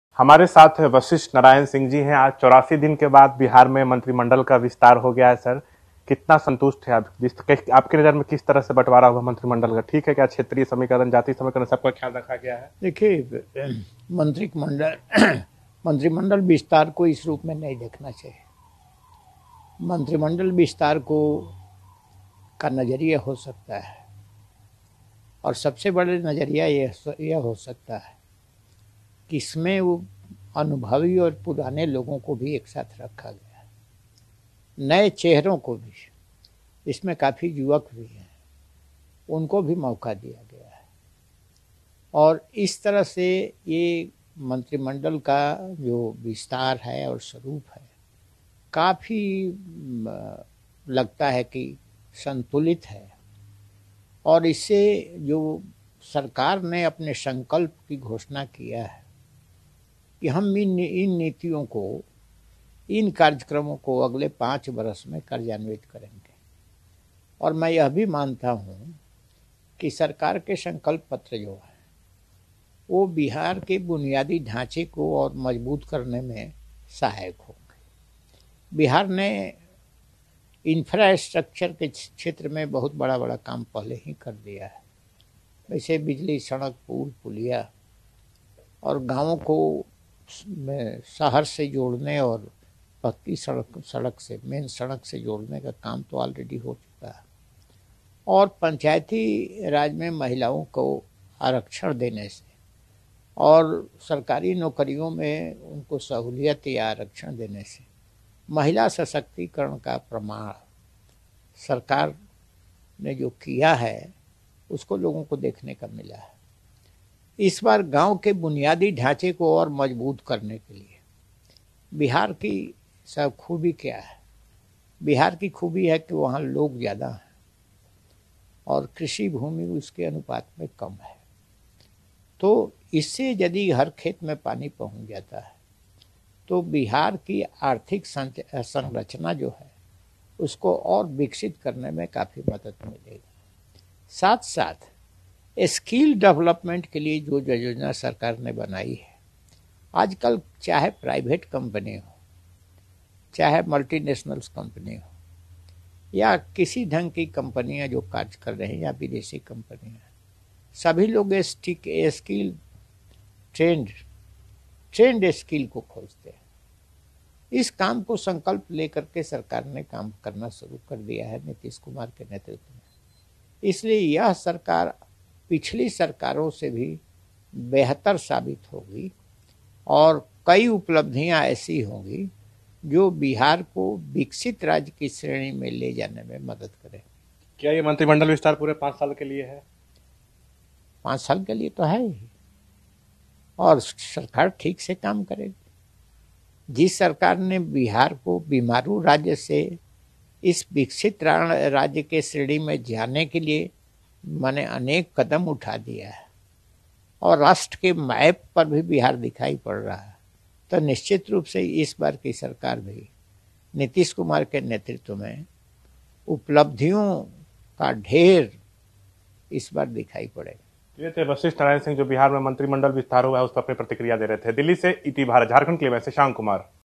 Balanced cabinet formed in Bihar: JDU Leader | ETV Bharat
Janata Dal (United) Rajya Sabha MP Vashishtha Narayan Singh said the Bihar cabinet wears a balanced look after it was expanded on Tuesday.